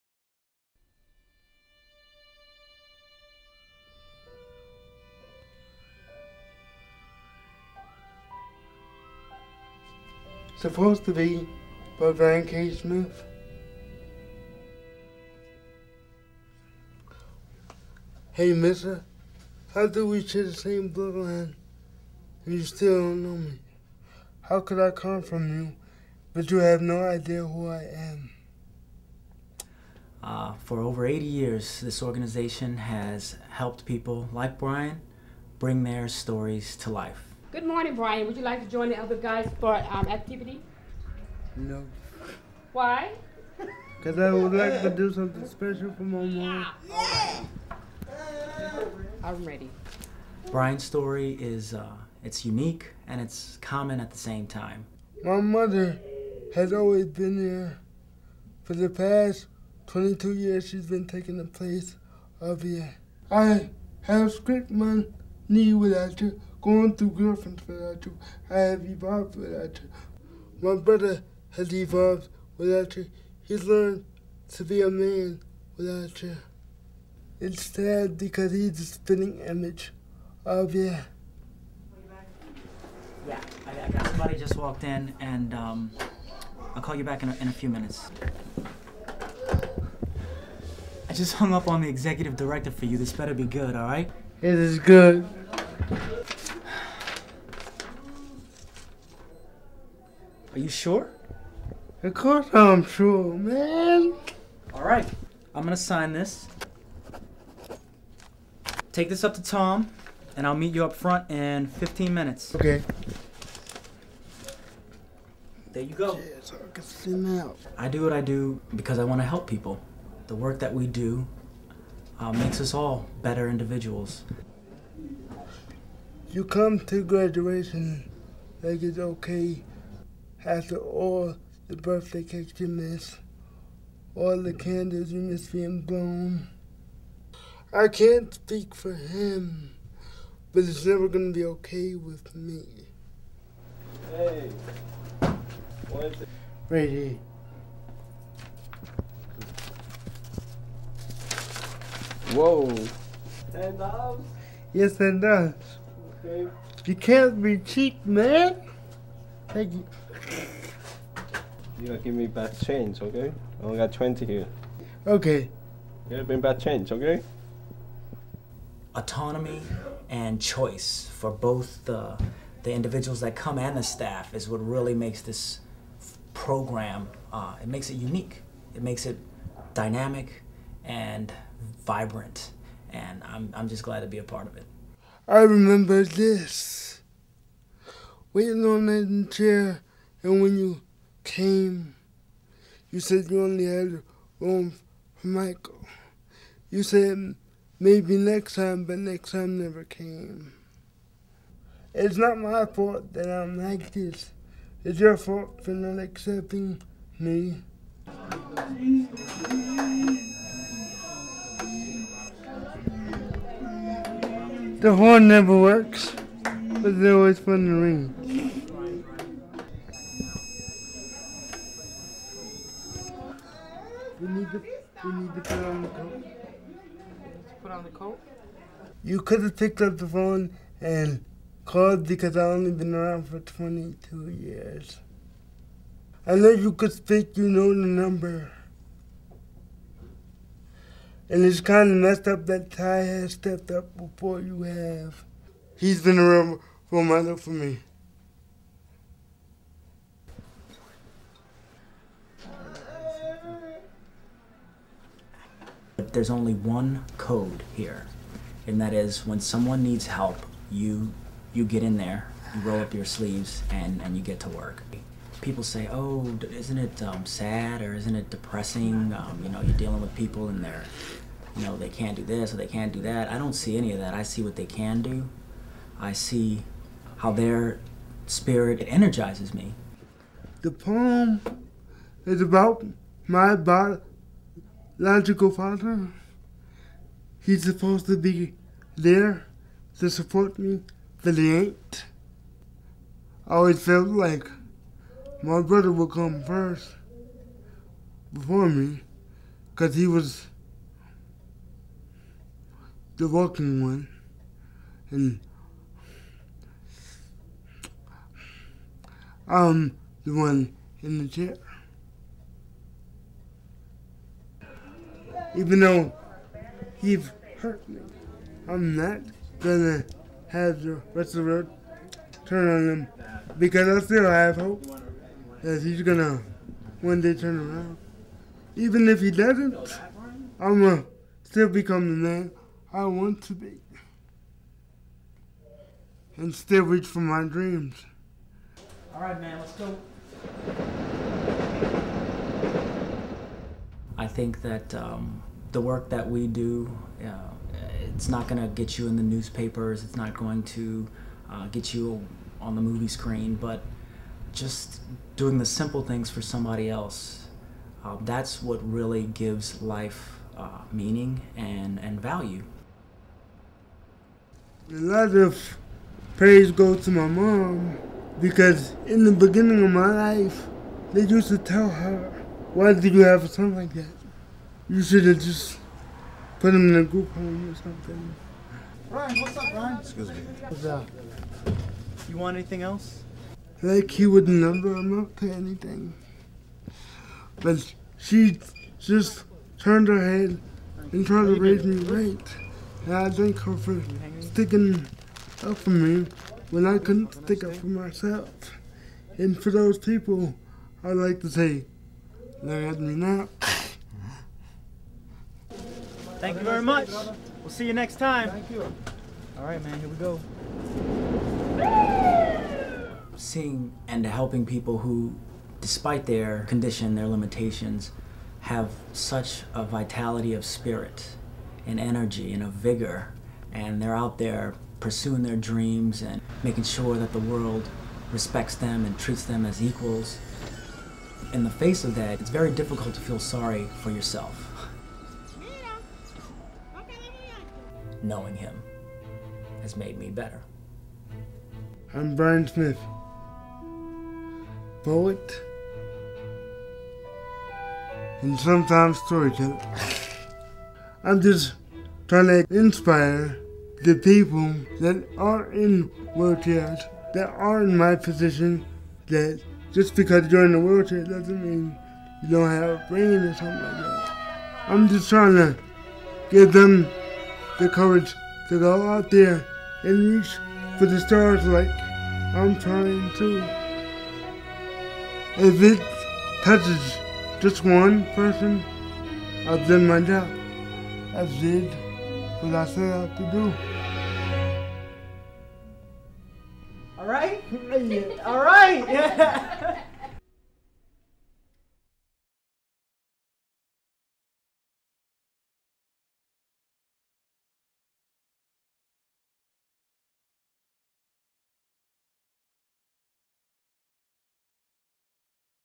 Supposed to Be
The Shield Institute presents "Supposed to Be", a poem by Bryan K. Smith